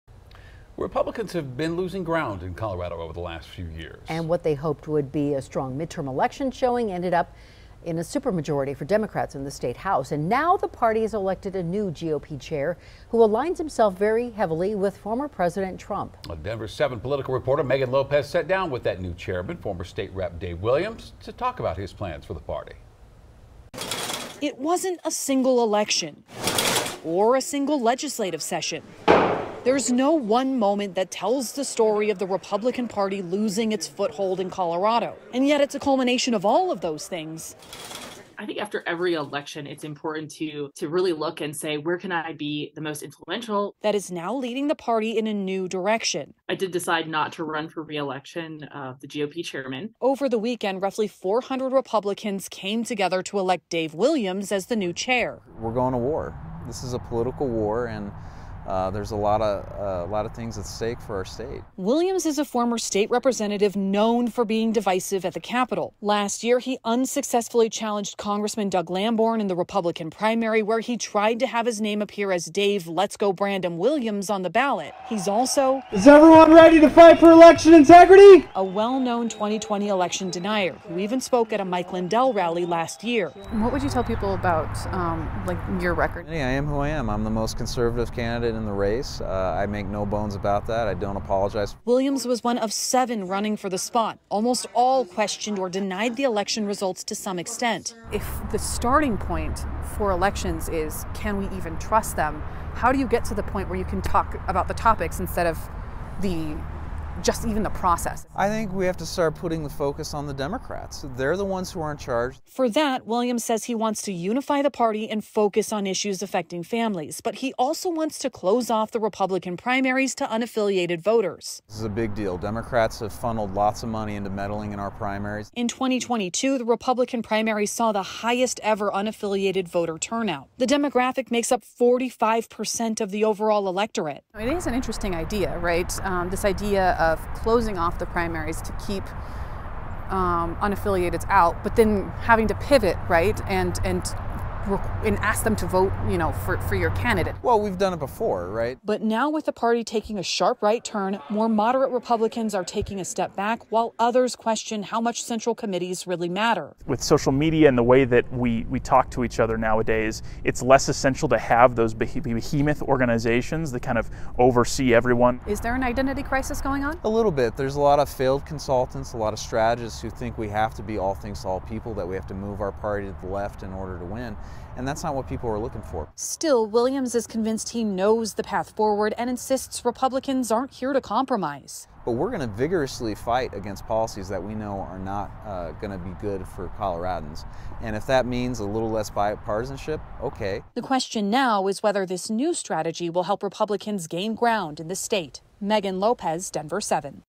Dave Williams becomes new Colorado Republican Party chair
Colorado Republicans elected a new chairperson to serve as the face of their party over the weekend. Dave Williams edged out six other candidates to take the top spot after several rounds of voting.